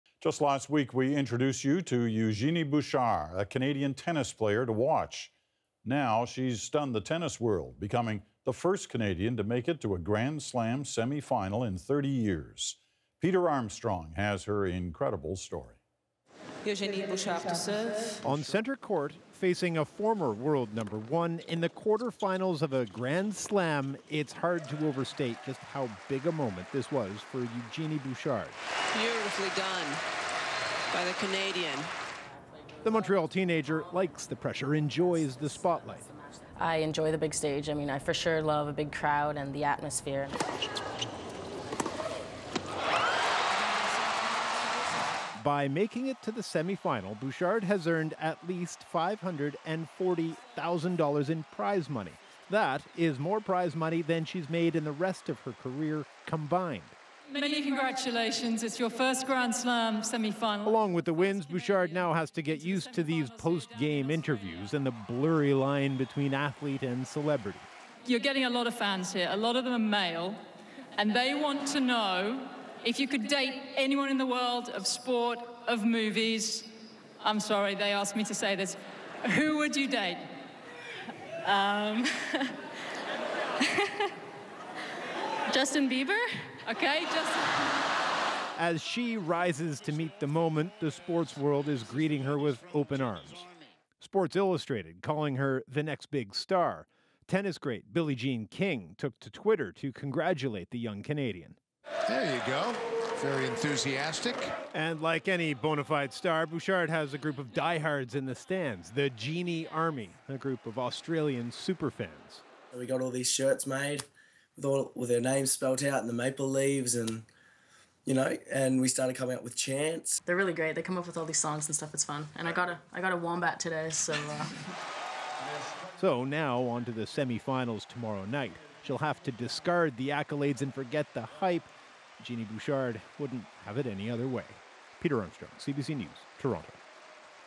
Eugenie Bouchard's big moment
Tennis player Eugenie Bouchard has become the 1st Canadian to make it to a grand slam semi-final in 30 years.